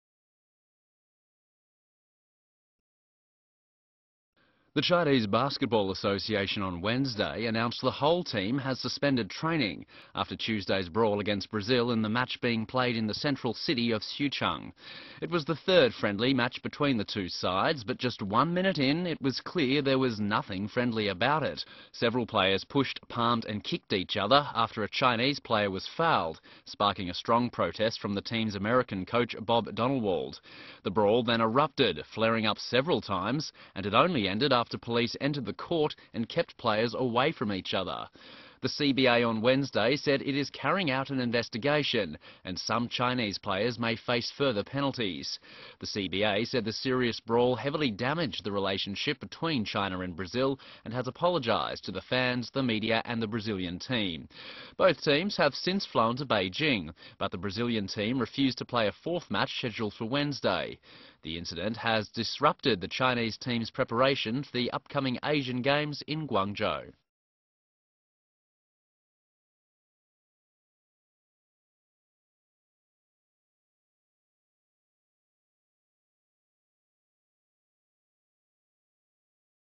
Basketball Brawl: China and Brazil Get It On
One minute into a game, China and Brazil enter into serious brawl.